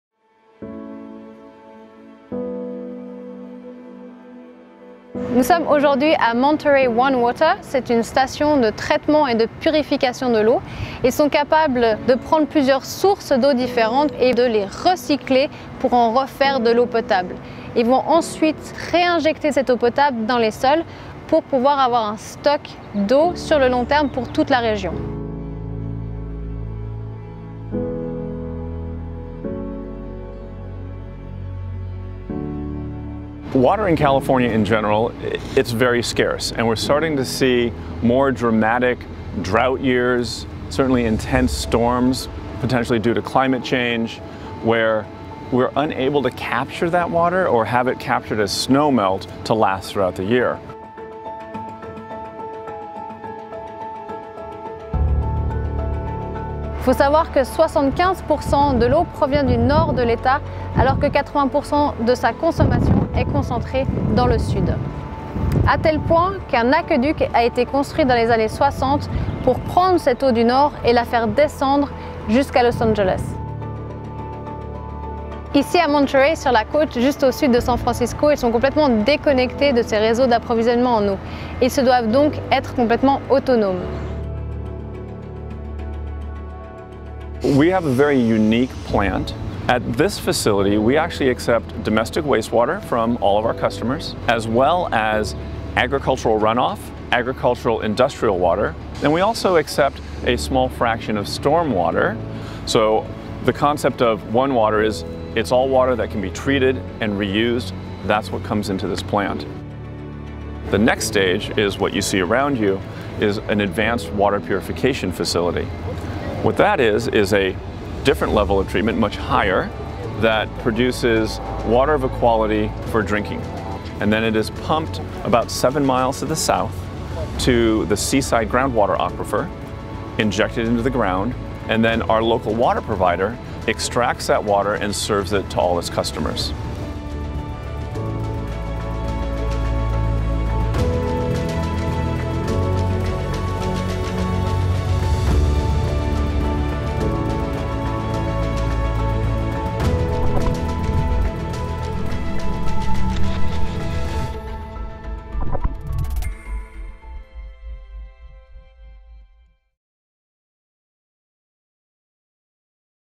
Le Traitement de l'eau en Californie - Journal de Bord 2021
La Californie doit faire face à un besoin en consommation d'eau élevé, plus de 39% de l'eau est utilisée pour l'agriculture. Katia Nicolet, notre scientifique embarquée et Agathe Roullin sont allées à Monterey One Water, une station de traitement et de purification de l'eau qui est capable de traiter et de stocker différentes sources d'eaux (eaux usées, eaux de pluie, eaux de l'industrie alimentaire) pour répondre aux besoins de la région.

Découvrez l'intégralité du sujet dans notre dernier Journal de Bord

⚙️ Activez les sous-titres en cliquant sur Paramètres !